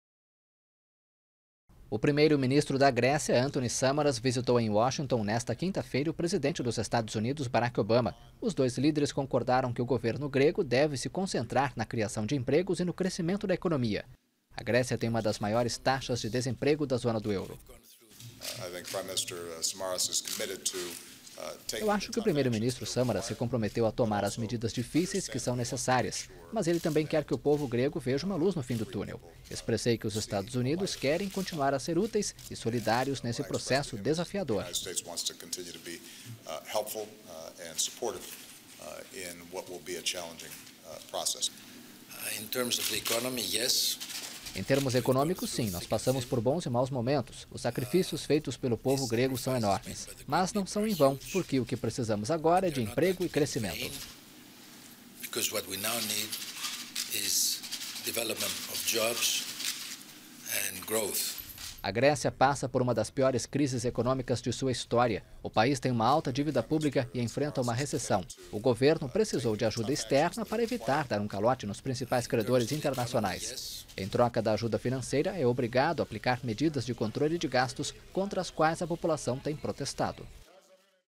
Grécia quer empregos e crescimento
O presidente americano Barack Obama recebeu nesta quinta-feira na Casa Branca o primeiro-ministro da Grécia, Antonis Samaras. Os dois líderes concordaram que o mais importante para o país Europeu, que enfrenta uma grave crise econômica, é se concentrar na geração de empregos e no crescimento.